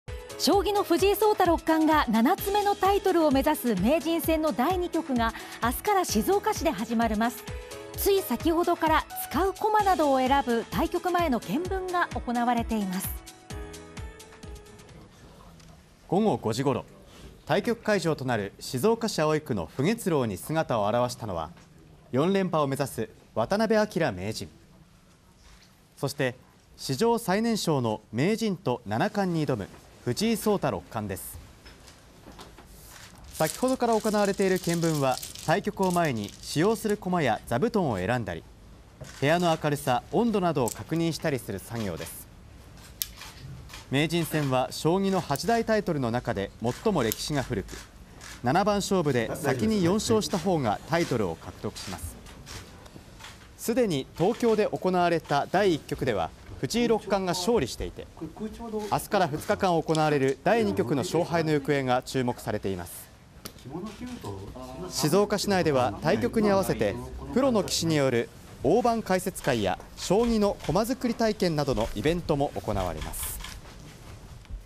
【藤井六冠が静岡市に】最年少七冠に挑む あすから将棋・名人戦 対局前の「検分」実施
将棋の藤井聡太六冠（２０）が７つ目のタイトルを目指す名人戦の第２局が４月２７日から静岡市で始まる。つい先程から使う駒などを選ぶ対局前の「検分」が行われた。